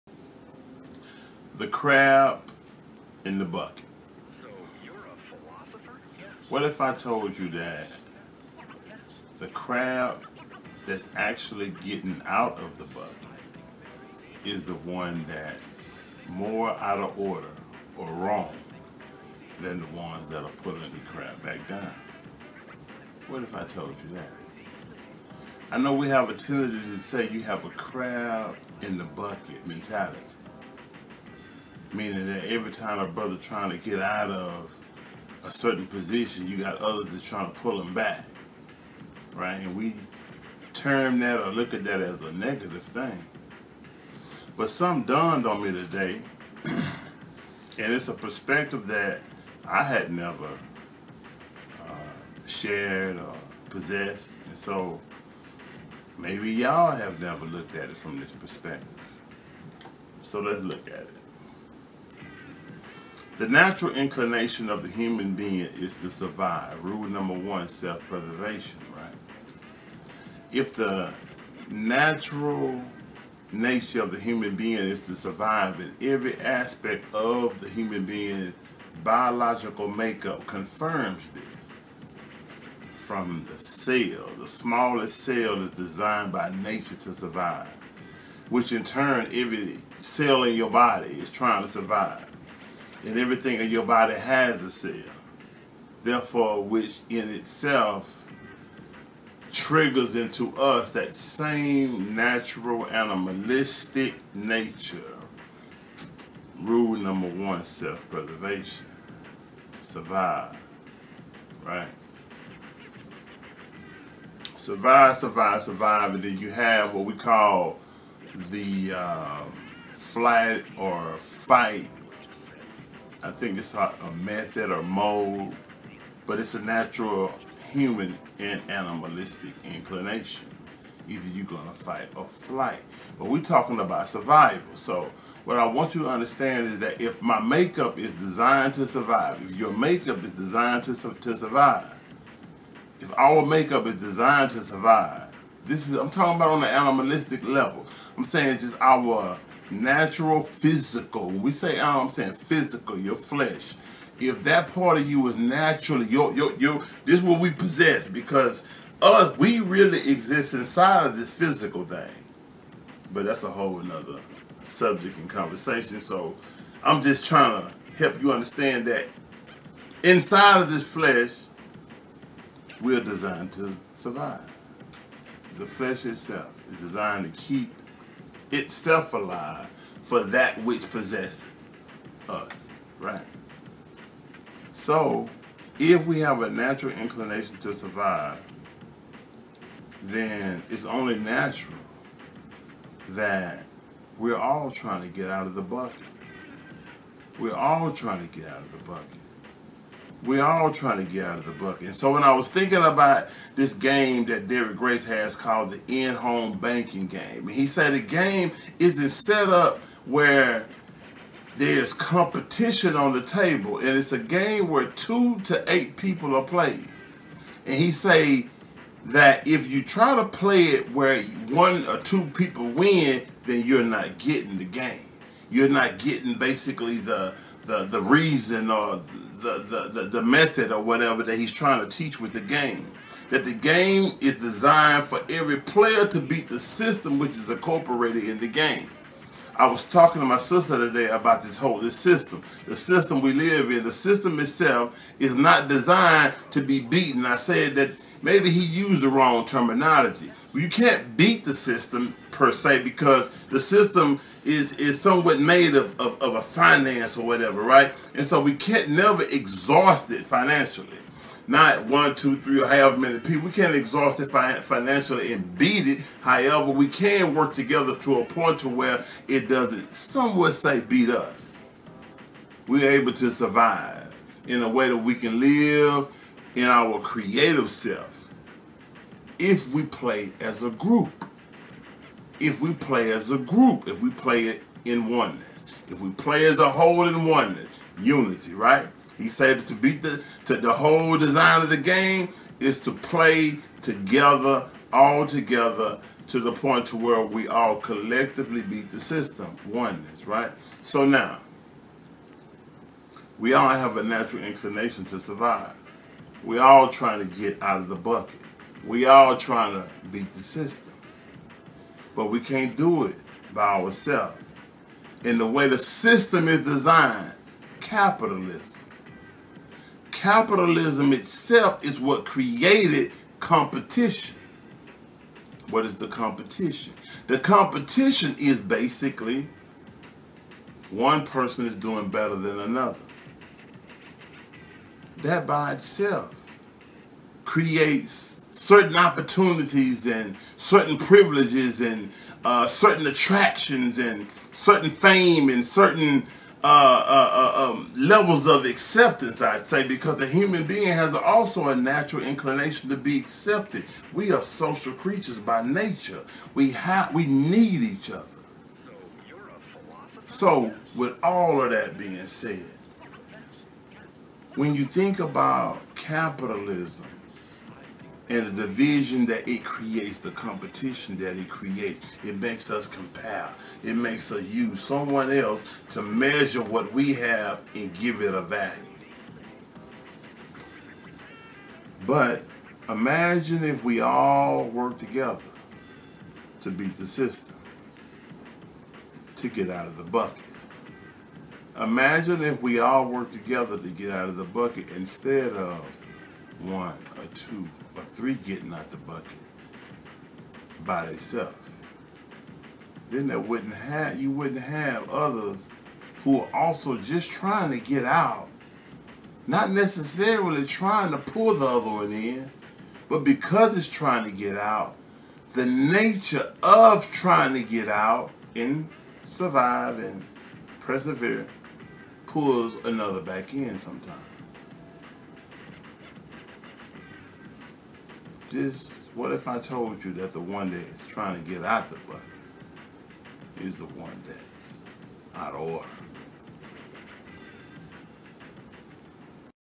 "Crabs In the Bucket: But what if I Told You..."
Crabs In the Bucket mentality! Other than trying to survive, trying to get out, what are they doing wrong?